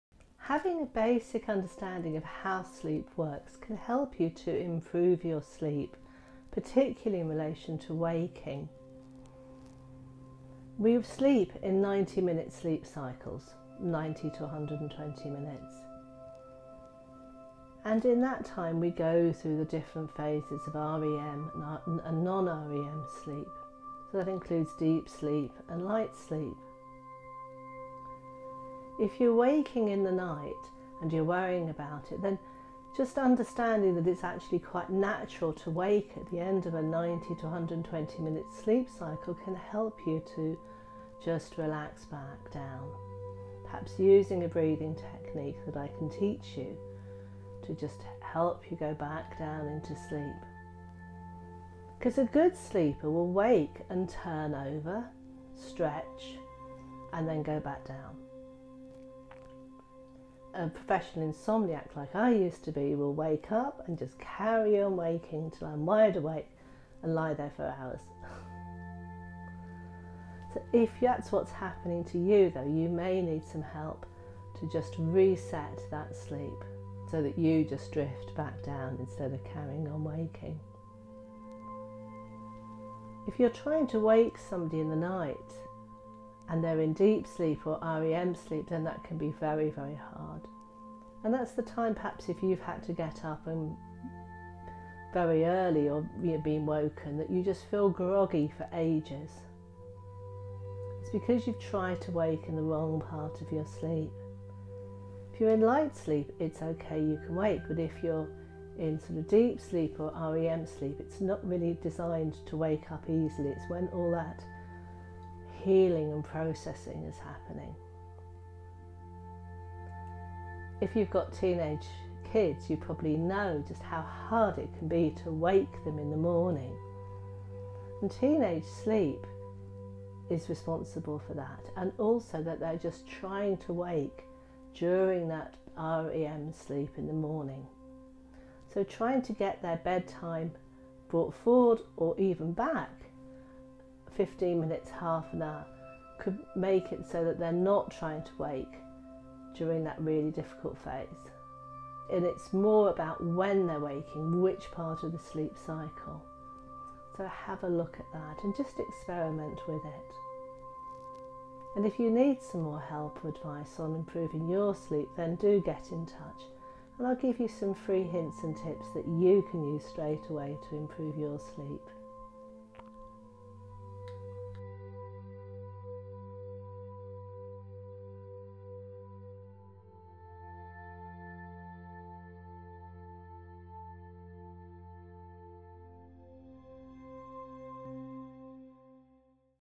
Do you wake in the night or struggle to wake in the morning? Understanding how sleep works can help.
Do wake up during the night or find it hard to wake in the morning?  Learning how sleep works  your sleep stages and your sleep cycle may help you to understand what is happening.  This video briefly explains sleep stages and sleep cycles.